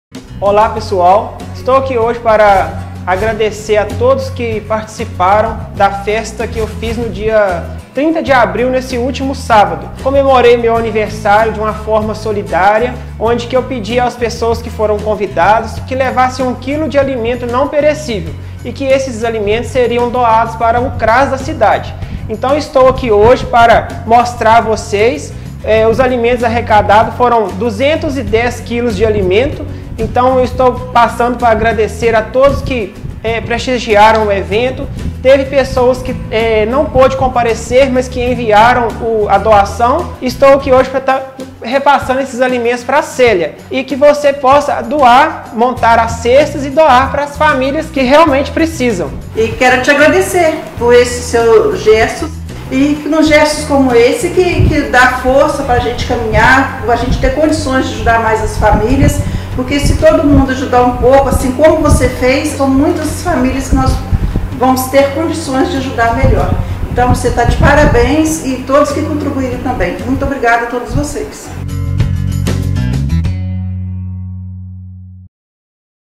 Doação de alimentos para a Assistência Social de Guidoval.
No último sábado (30/04), realizou-se em Guidoval, na comunidade Vargem Alegre, um evento beneficente, "Aniversário do Cristian Melo", o atual Secretário de Governo da administração municipal (Sr. Cristian), arrecadou 210 quilos de alimentos como doação dos convidados do evento, e repassou a doação para a assistência social do município, que logo estará distribuindo em forma de cestas básicas para as famílias que precisam em Guidoval.

São ações assim que possibilitam que possamos ajudar mais famílias em nossa cidade.

Parabéns ao Sr. Cristian Melo e a todos os envolvidos nesta doação.